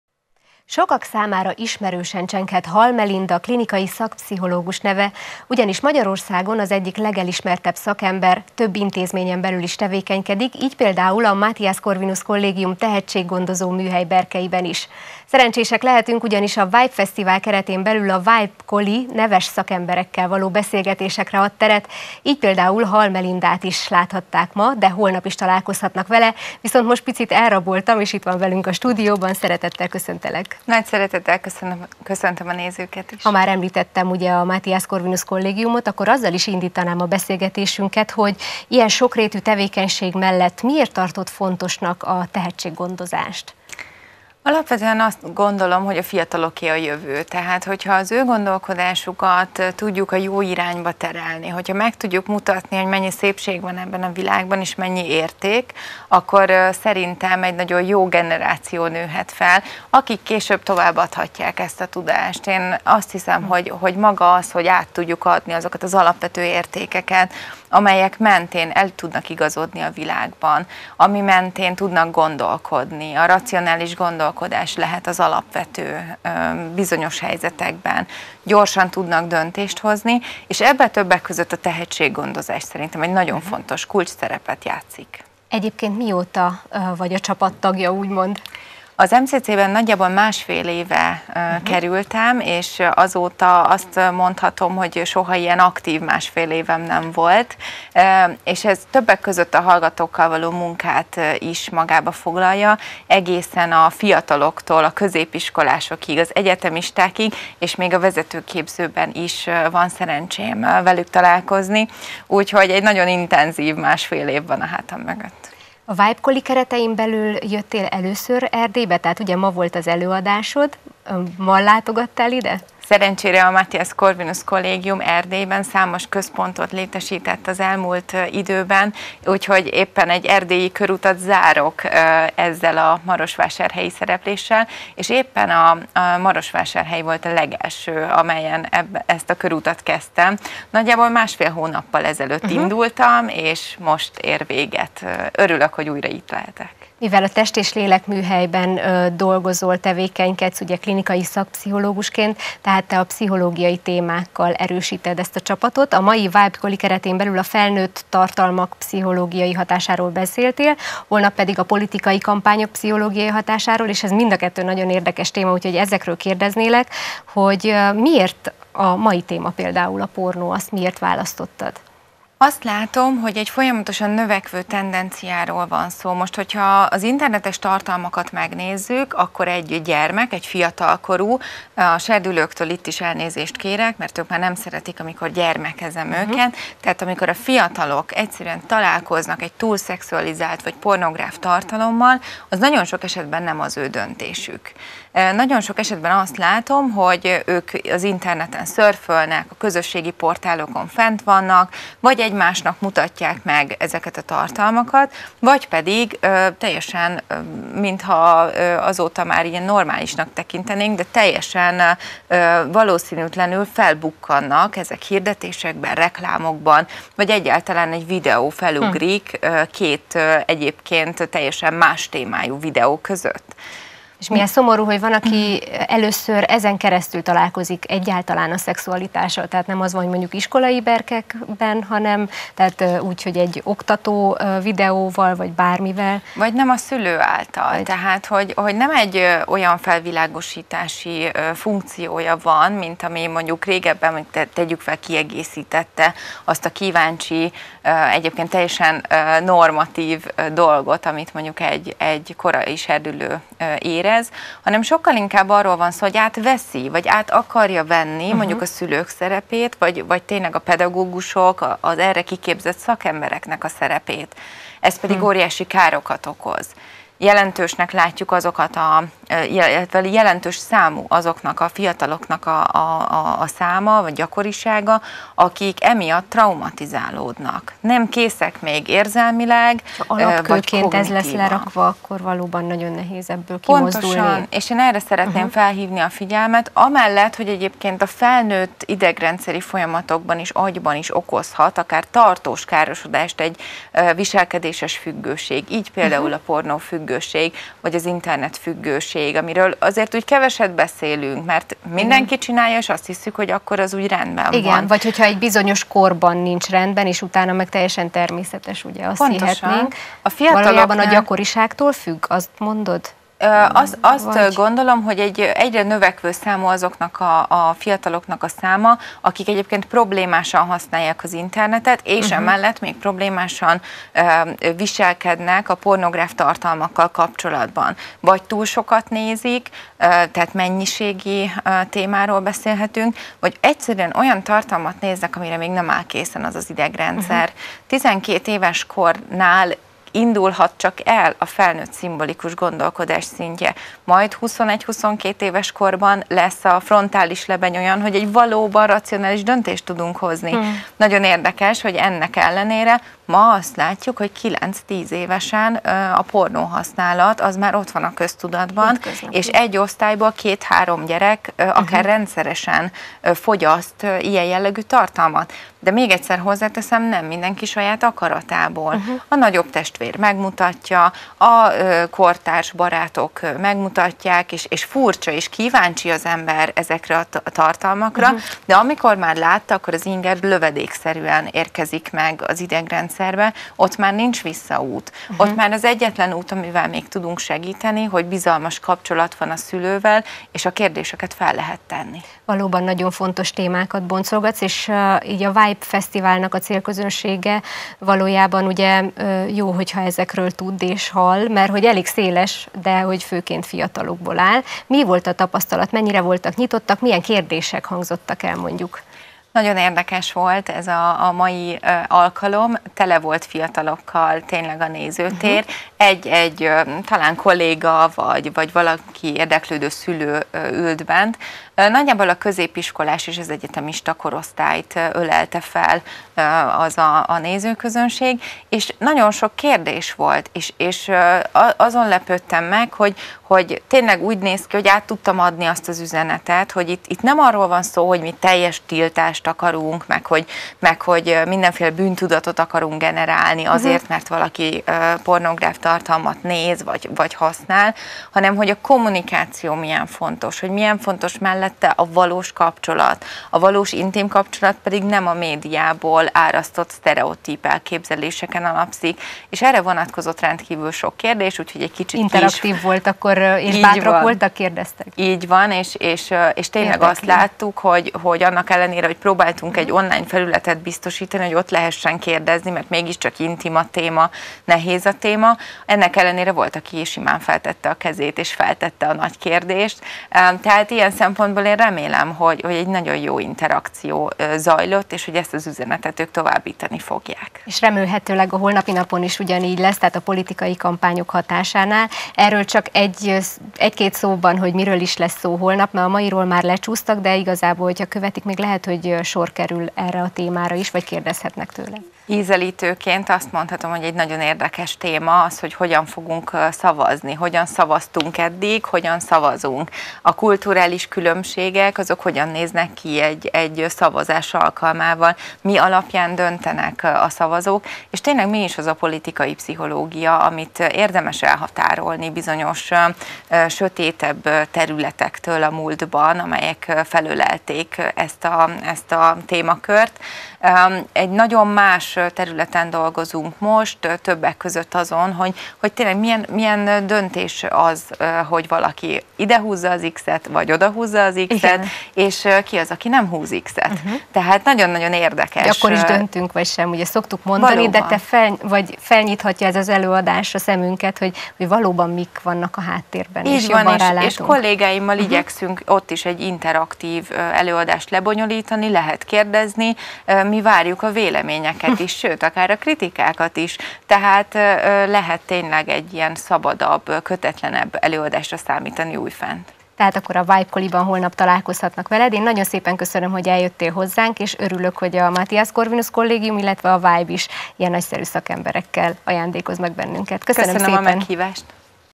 Befolyásolhatóak vagyunk? – Mozaik
⭐ A Mozaikban a tehetséggondozás fontosságáról beszélgettünk Hal Melinda klinikai szakpszichológussal, aki a Mathias Corvinus Collegiumnál is tevékenykedik.

🔞 A beszélgetésben szó esett a felnőtt tartalmak és a politikai kampányok pszichológiai hatásairól is.